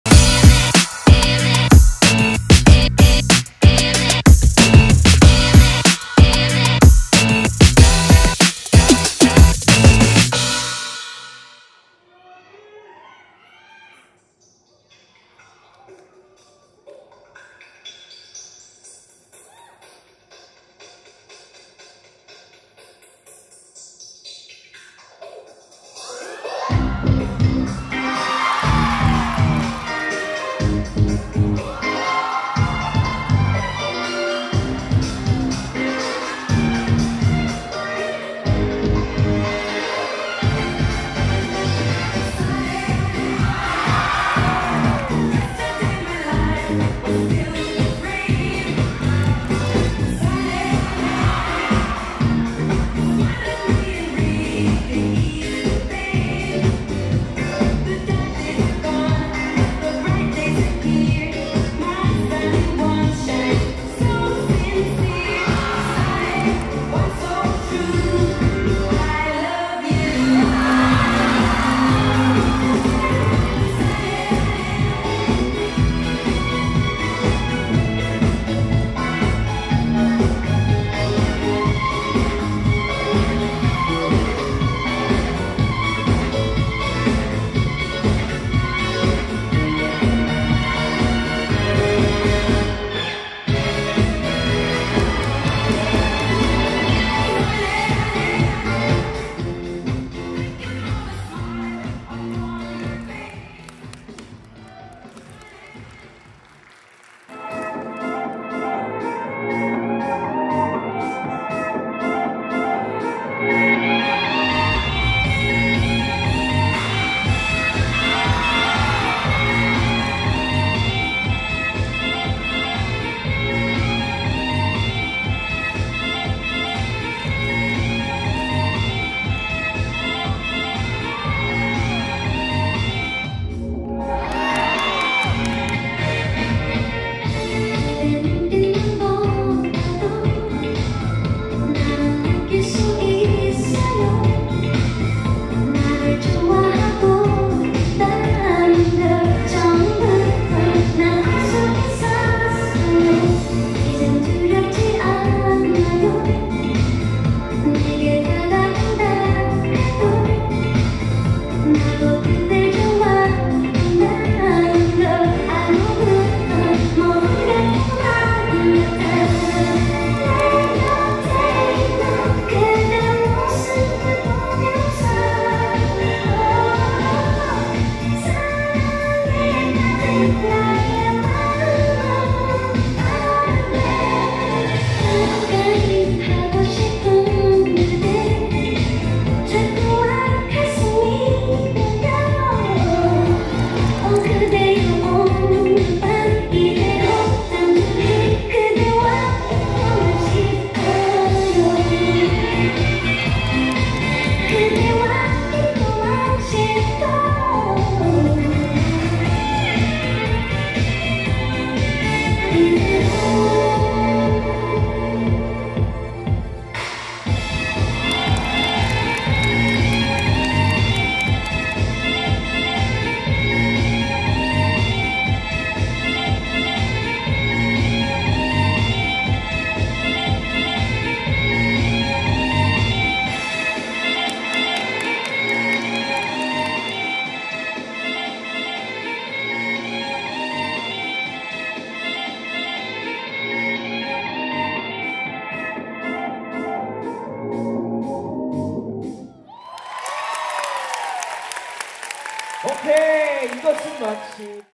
BIBI STAGE | TOPVIEW | 2023 FEEDBACK STAGE | 2023 피드백스테이지
2023 FEEDBACK STAGE
“2023 피드백 스테이지”

CHOREOGRAPHER STAGE
“BIBI”

PRESENTS
FEEDBACKSTUDIO
(주)피드백스튜디오

DIRECTOR
SO

SUB DIRECTOR
JUFILL

후원: CRITIC, FEEDBACKDANCESTUDIO
운영도움: FLOWMAKER
촬영: Daniel Song / JINKY / Na Yeeun